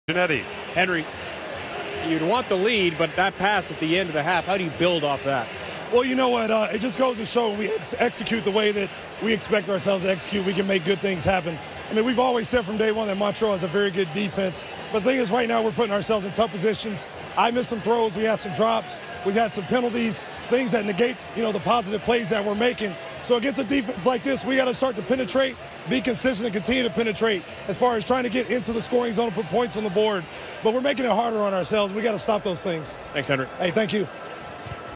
August 19, 2016 - Henry Buris half time TSN interview
August 19, 2016 - Ottawa vs. Montreal